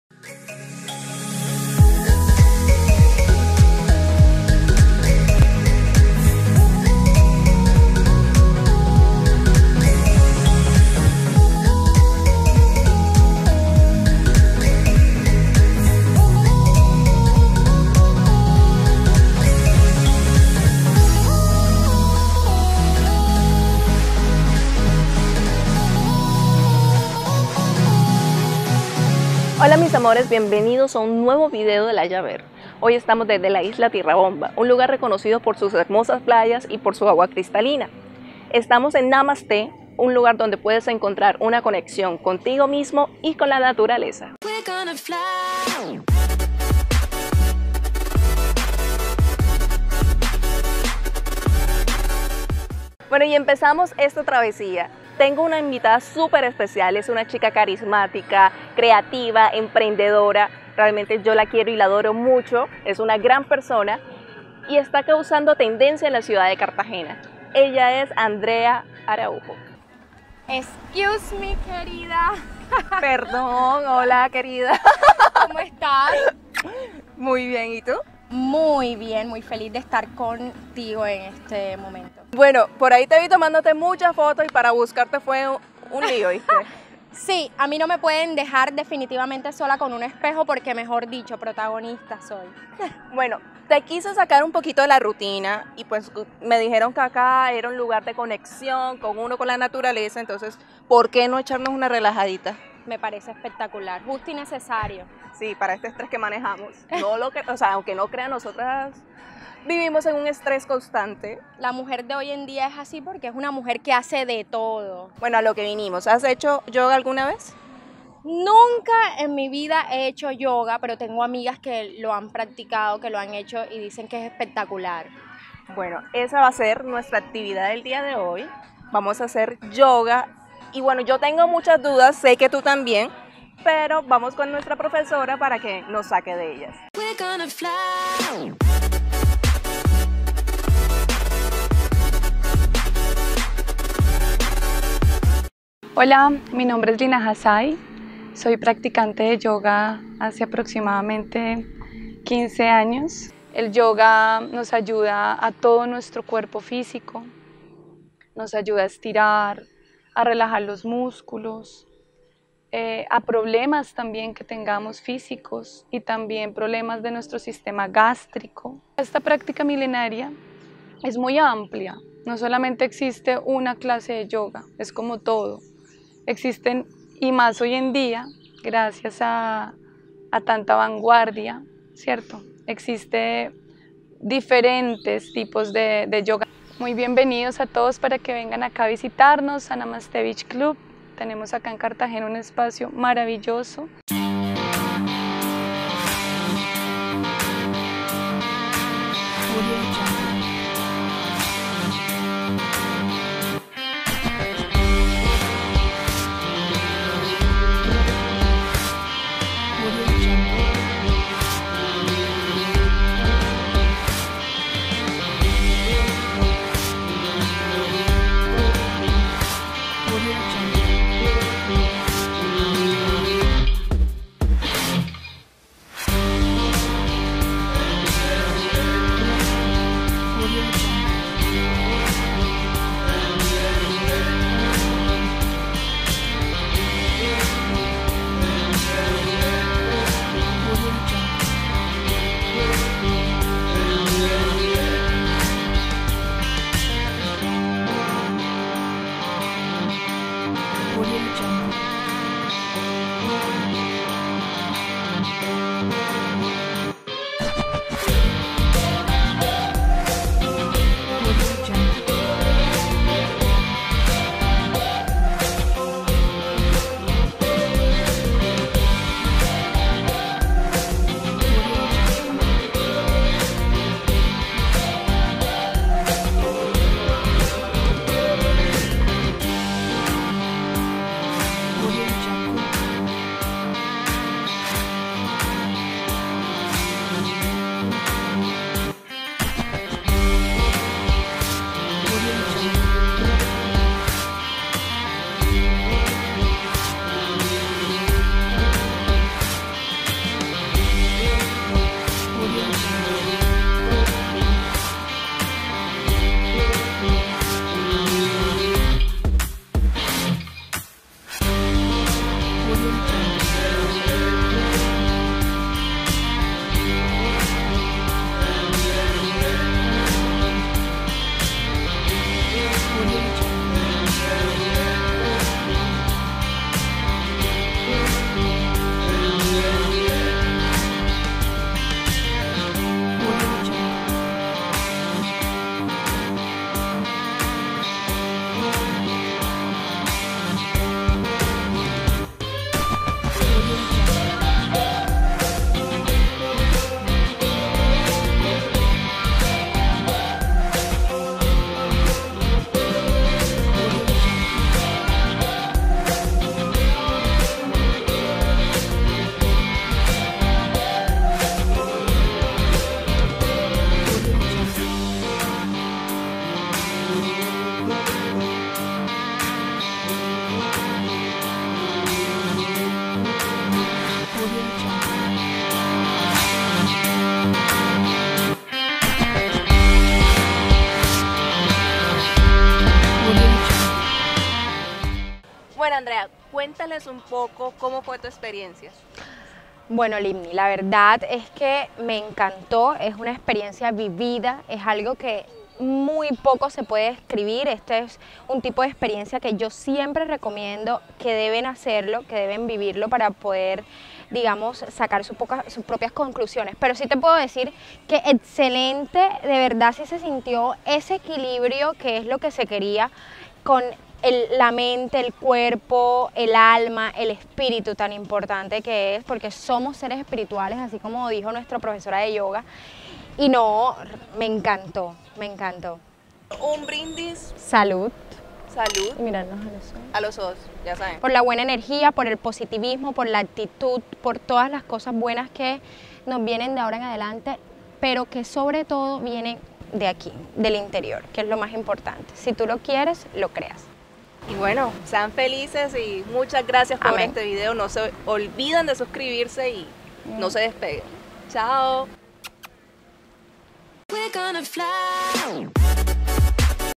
"La Yaber": Una de las mejores experiencias REALIZANDO YOGA!!!
Esta experiencia fue increíble, la realicé en Namaste un hotel ubicado en la isla Tierra Bomba, en la ciudad de Cartagena de Indias, en compañía de Andrea Araujo una joven empresaria y aventurera.

El lugar perfecto para descansar, acercarse al mar y encontrarse con uno mismo.

Instagram de la locación: @namastebeachclub
Instagram del canal : @La.yaber